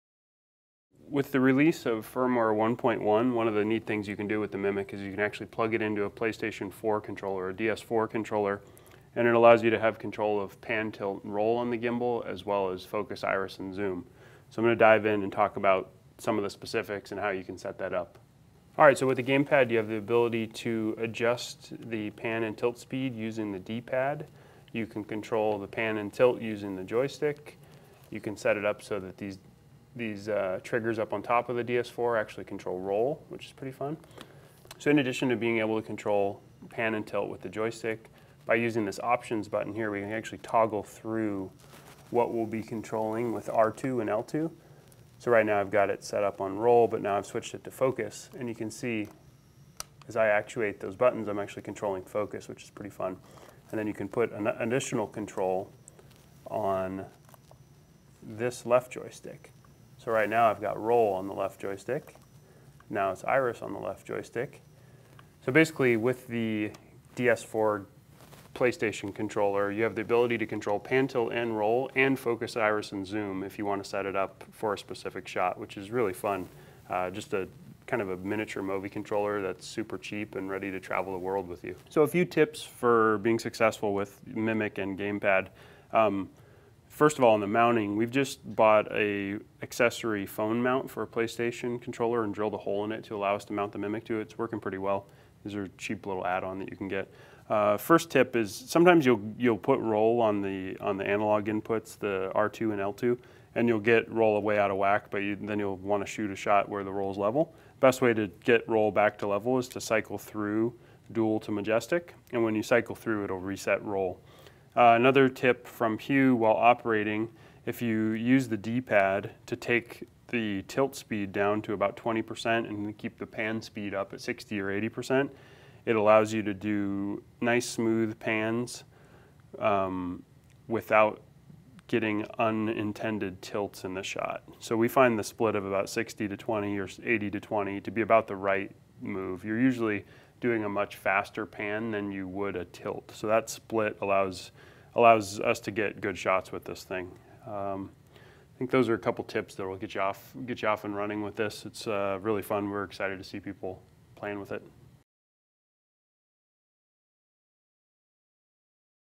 MōVI PRO - MIMIC Gamepad
A overview of Freefly MōVI Pro MIMIC gamepad control using a Playstation DS4 controller.

Playstation DS4 controller
DS4 controller mount